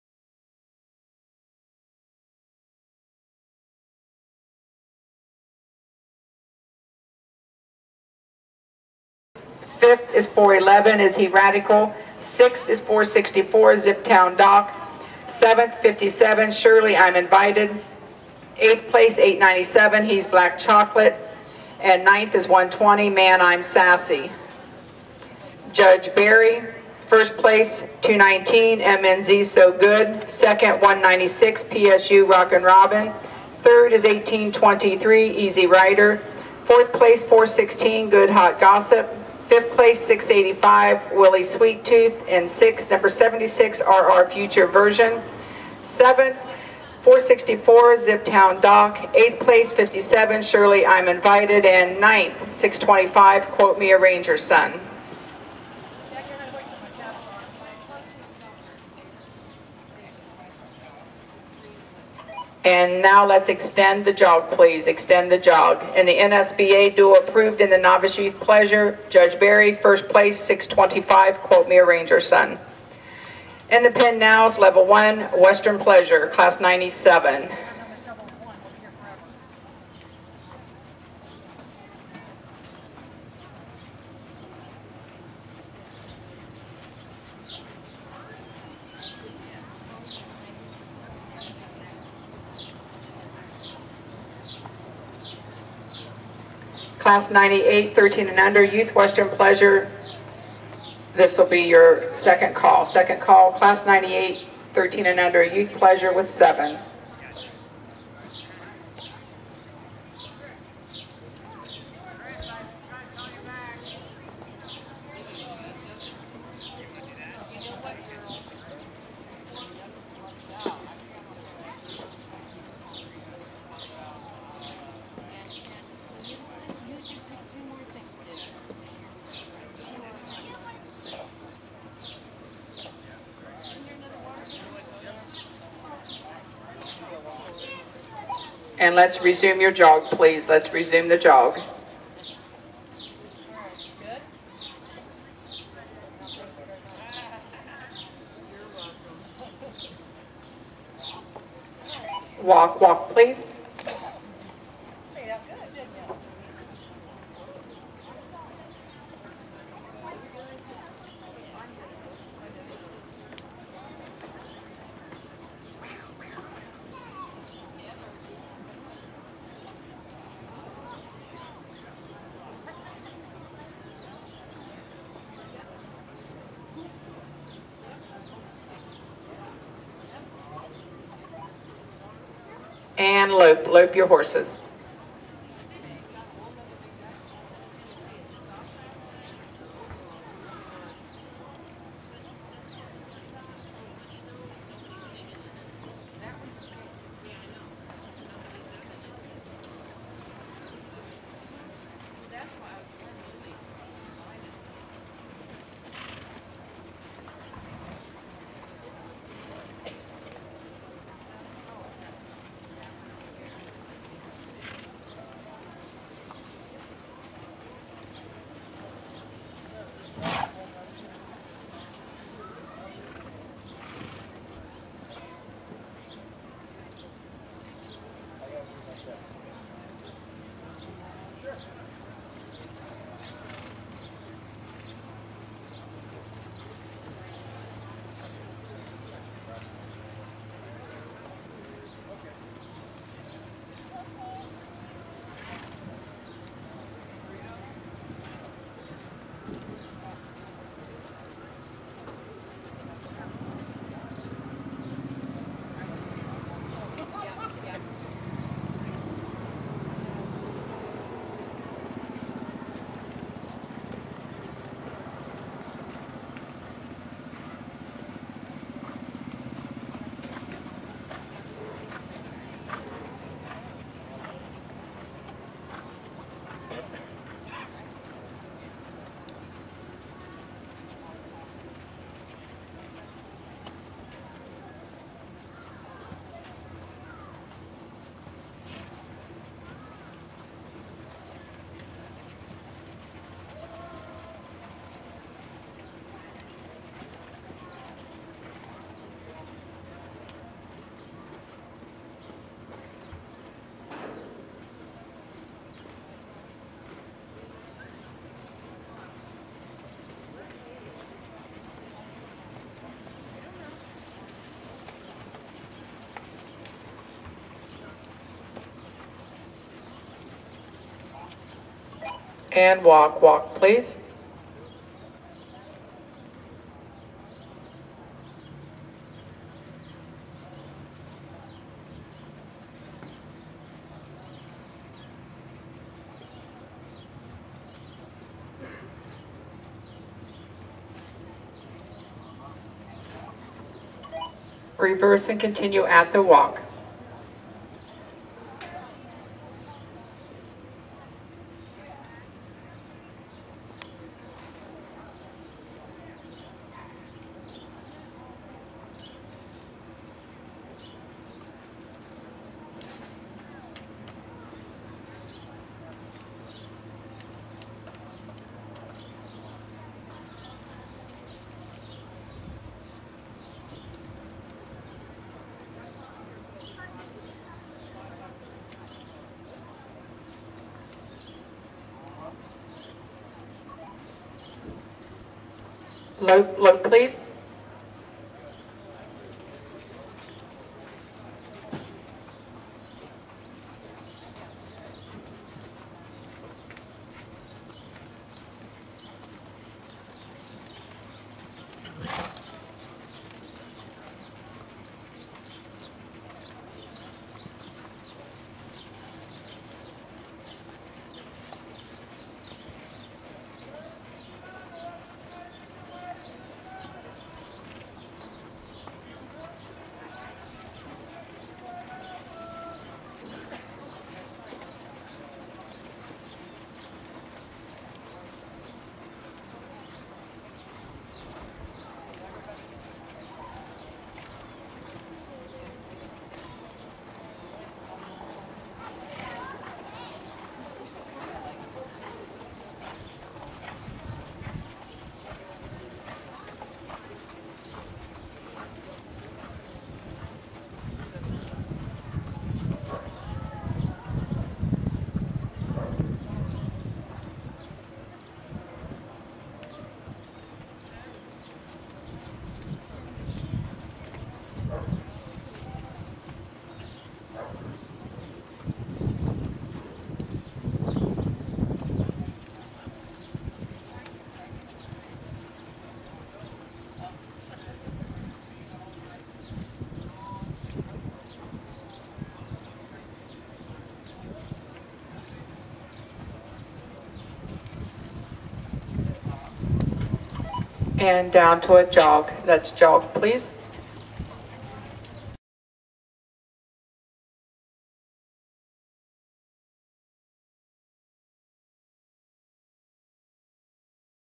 SOLD...2004 AQHA & APHA Double Registered Gelding by Too Sleepy To Zip
SOLD...2004 AQHA & APHA (spb) Western All-Around Gelding. Sired by Too Sleepy To Zip and the dam is out of Invitation Only.

Bob is currently 6th in the nation (as of 8-24-14) in AQHA Sr Green Western Pleasure
and was the 2013 Ohio High Point Novice Am Western Pleasure Horse. He has over 80 AQHA Western Pleasure points and 8.5 AQHA Trail points. He has been started in Showmanship and Horsemanship. He also has a lead change.

Very Reasonably Priced